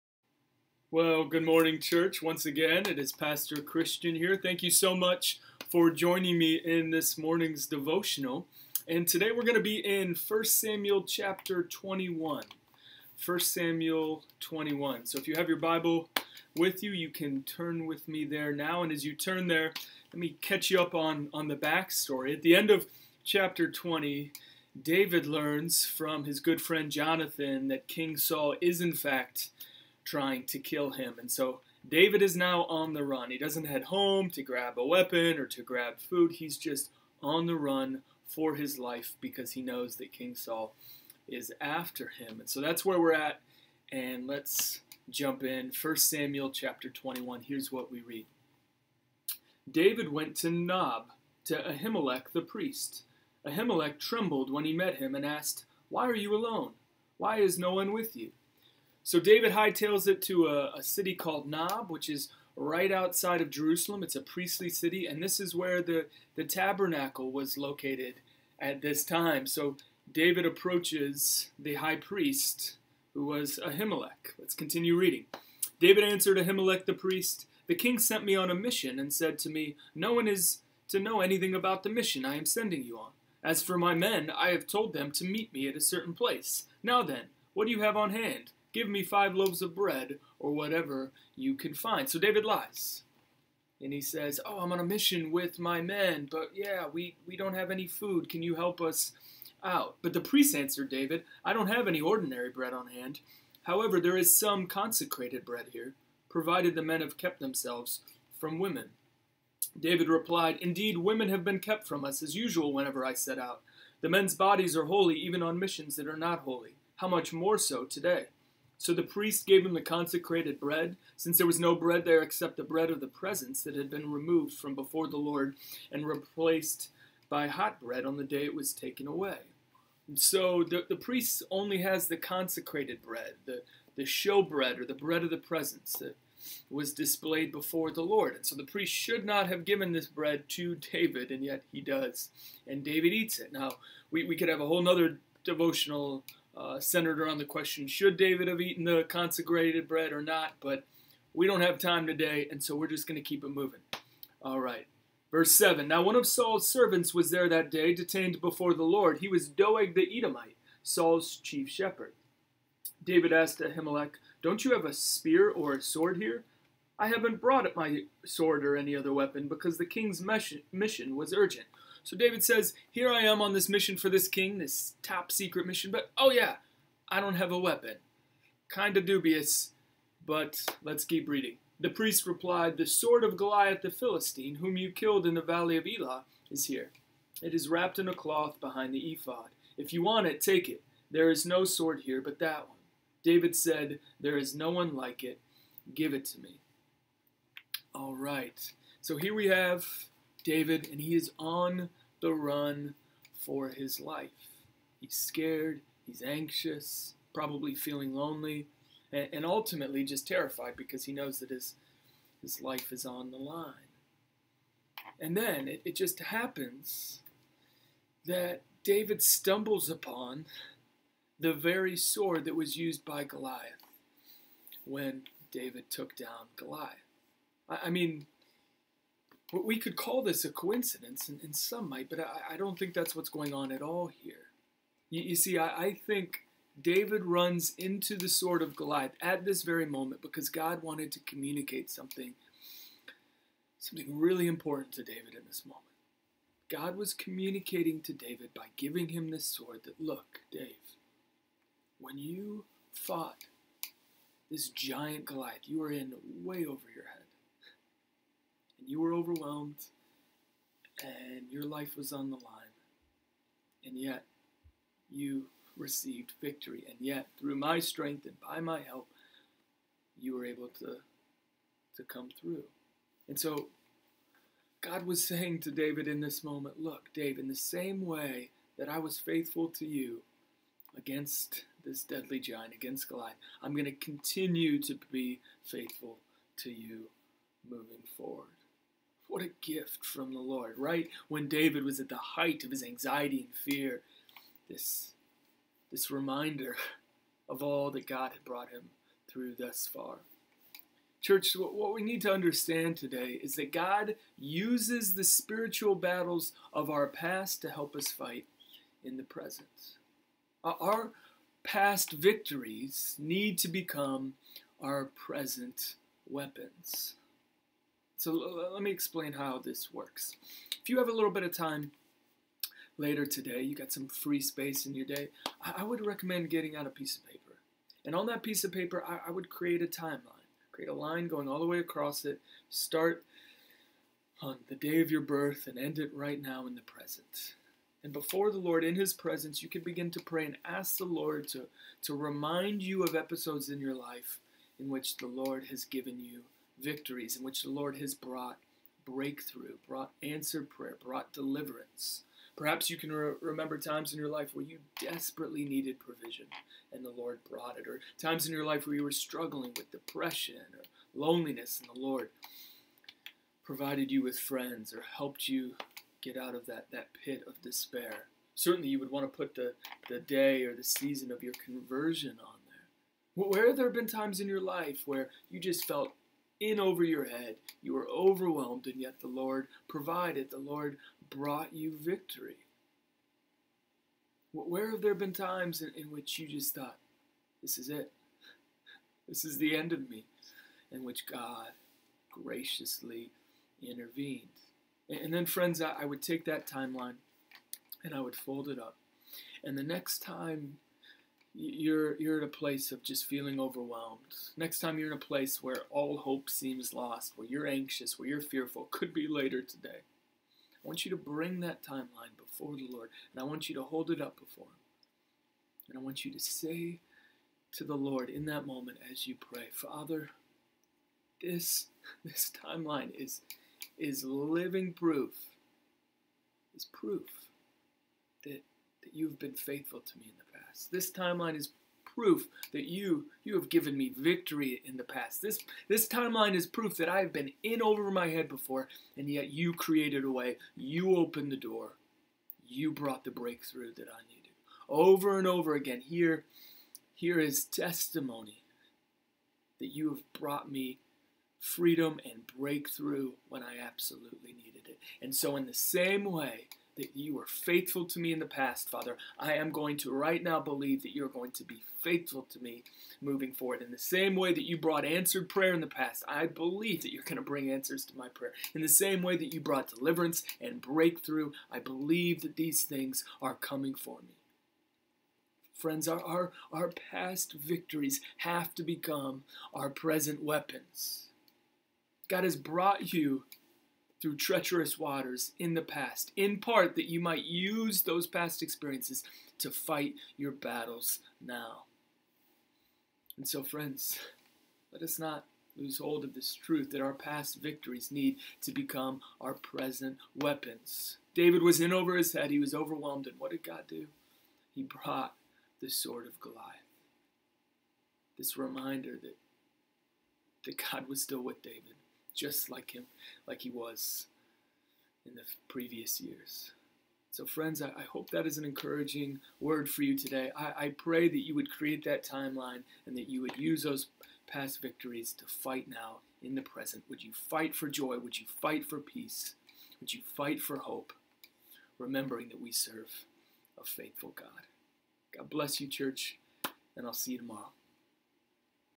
Daily Bible Devotional with Pastor Christian Root.
Pastor Christian Root of Grove City Vineyard Christian Fellowship brings today's devotional out of 1 Samuel 21.